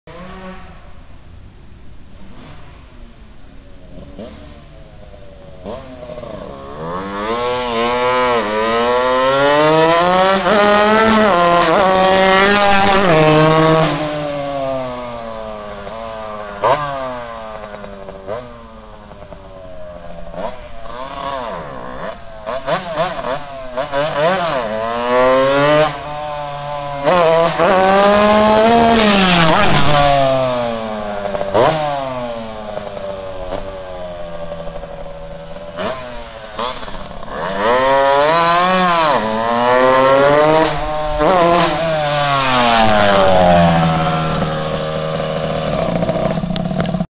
71 G3SS with CR85 motor testride
71 kawasaki G3SS with 02 Cr85 motor. Gearing way off. need to drop front sprocket 1 tooth minimally, probably 2. Still needs thorough tuning but so far no issues.